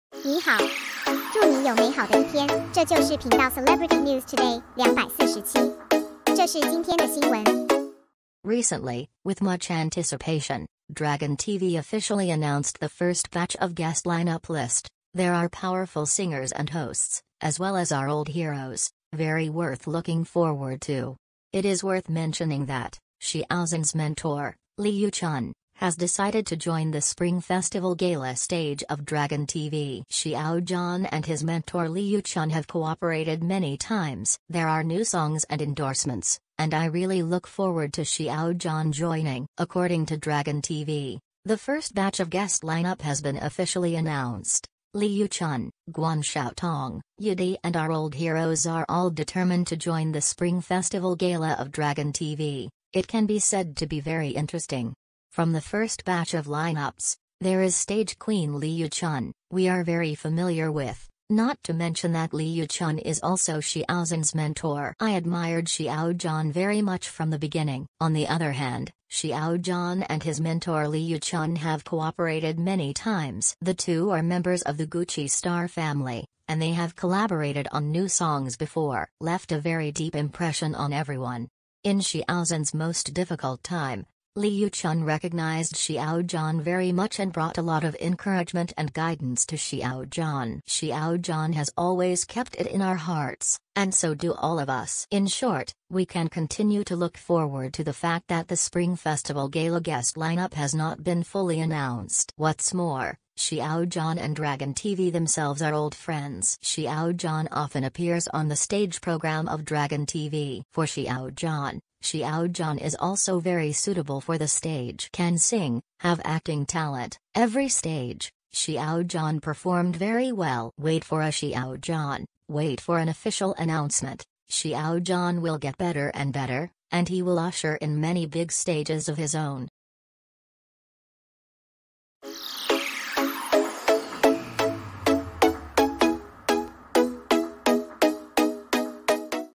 "Dragon TV" Spring Festival Gala lineup officially announced, Xiao Zhan's mentor confirmed to j...
Celebrity News Today 247 2022/01/21 : "Dragon TV" Spring Festival Gala lineup officially announced, Xiao Zhan's mentor confirmed to join, the two have cooperated many times ...
 Recently, with much anticipation,  "Dragon TV" officially announced the first batch of guest lineup